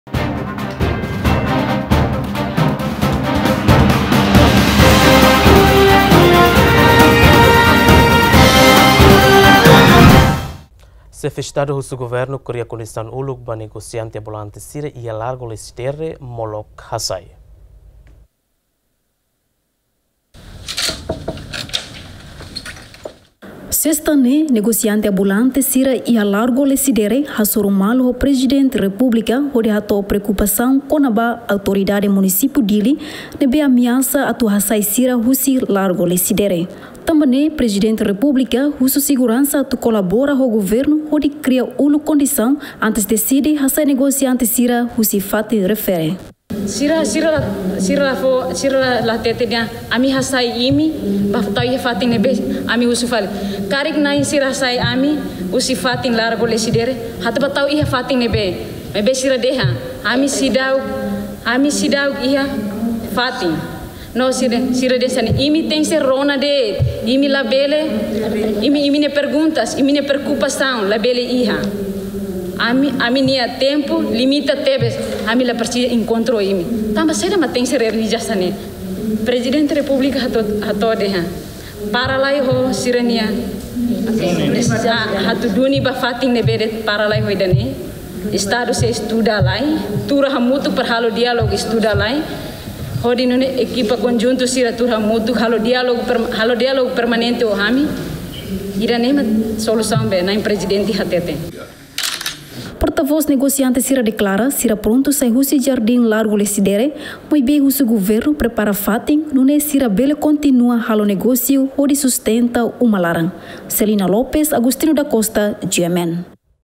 PR Horta: Kria kondisaun uluk ba Negosiante Ambulante iha Largo Lecidere molok hasai
PR Horta husu Governu kria kondisaun uluk ba Negosiante Ambulante Iha Largo Lecidere molok hasai